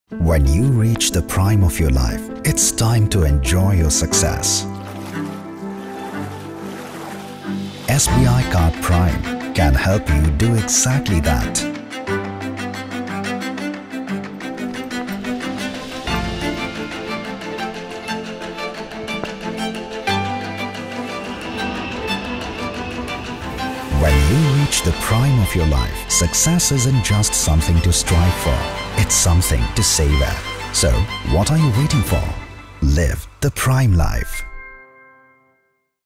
Live the PRIME Life | SBI Card PRIME
Immerse yourself in a beautiful world where thoughtful benefits are seamlessly combined with indulgent rewards and premium memberships.

With your SBI Card PRIME, you can now enjoy ₹ 3,000 Welcome Voucher, complimentary memberships to Trident Red Tier, Club Vistara Silver & Priority Pass Program, 5X Reward Points and much more.

T&C Apply